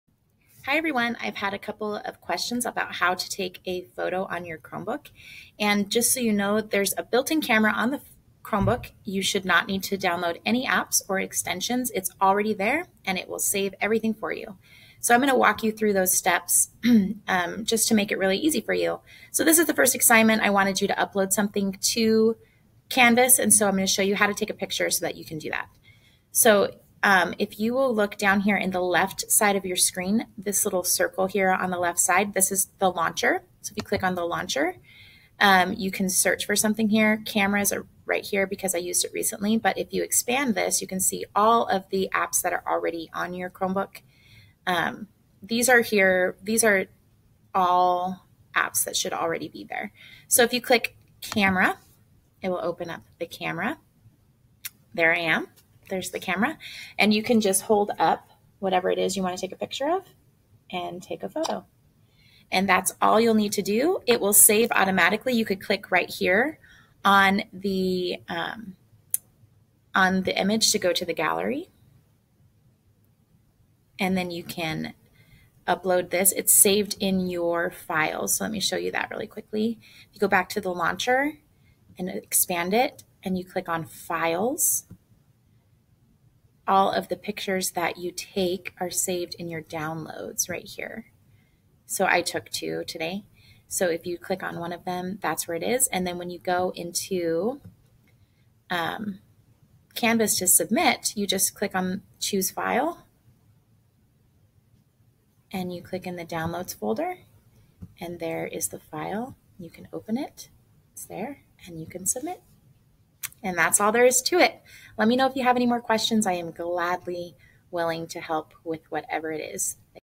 How to Take a Photo on the Chromebook
Ms. Cottman, our robotics facilitator, walks you through the steps to use your Chromebook's camera to take pictures and upload them to a Canvas assignment.